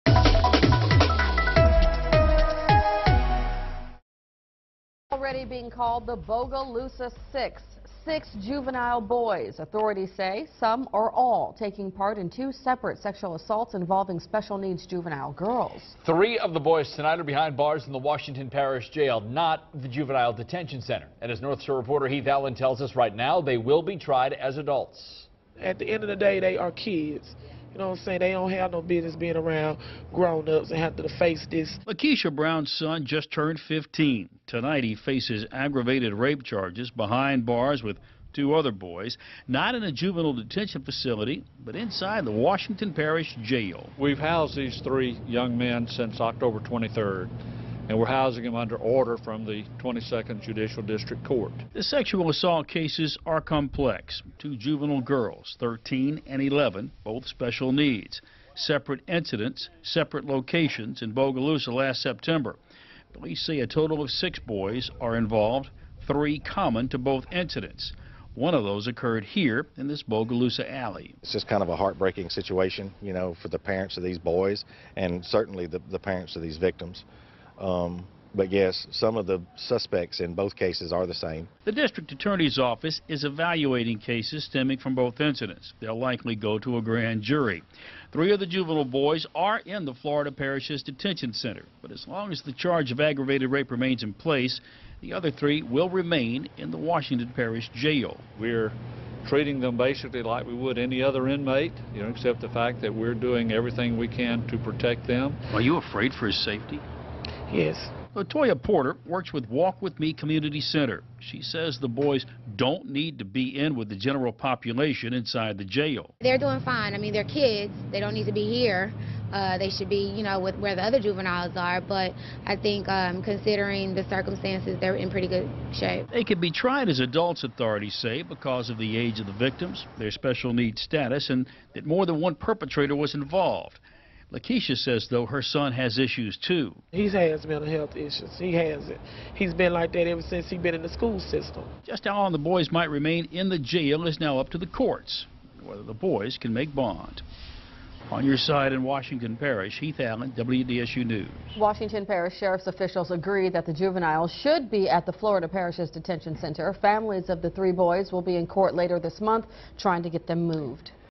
Juveniles held in Washington Parish jail in sexual assault investigation
Three juveniles are being held in connection with a sexual assault case in the Washington Parish jail.